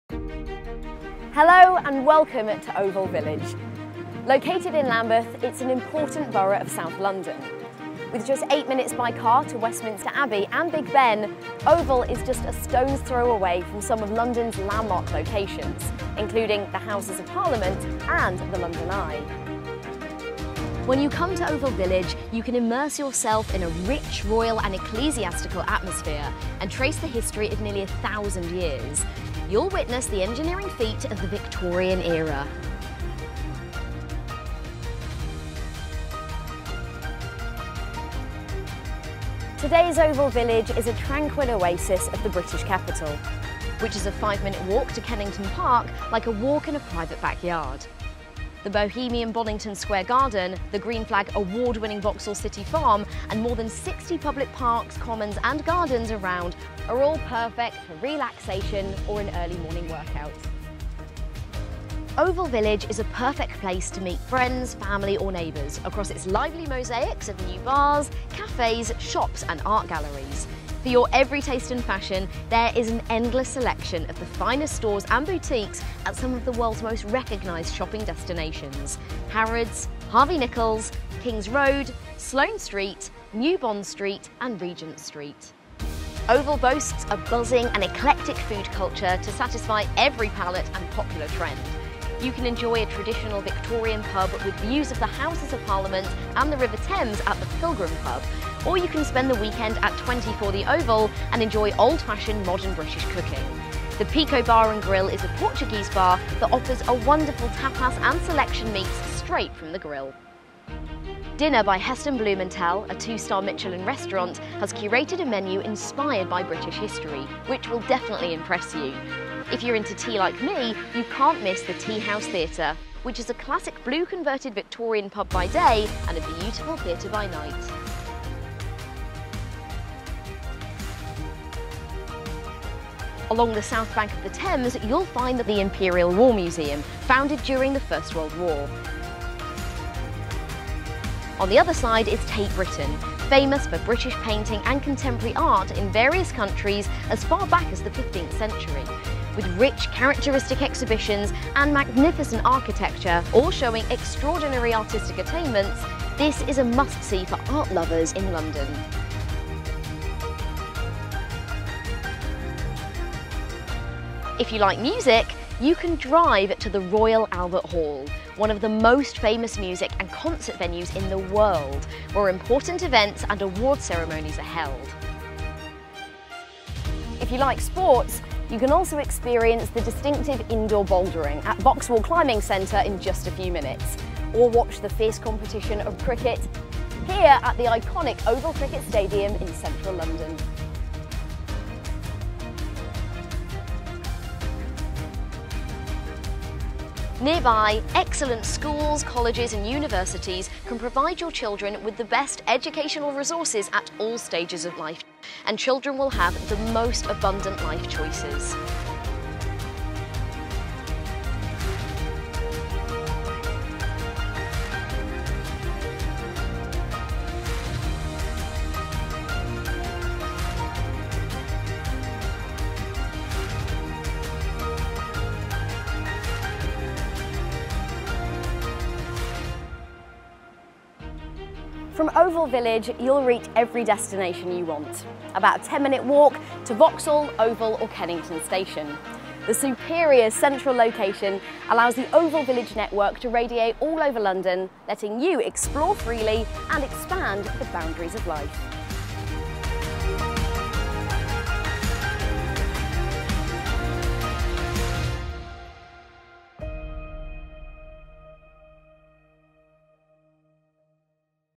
Oval Village Local Area Film
Gas Film x Berkeley Group Plc

Oval Village Local Area film - English version

Developed by St Edward, Oval Village is located in zone 1, less than a 10-minute walk away from three tube stations, and surrounded by green space and award-winning parks.

Oval Village is positioned next to the famous Oval Cricket Ground, a short walk from the River Thames. In this Local Area Film of Oval Village, you can see the trendy shops, bars and restaurants around this outstanding new property development.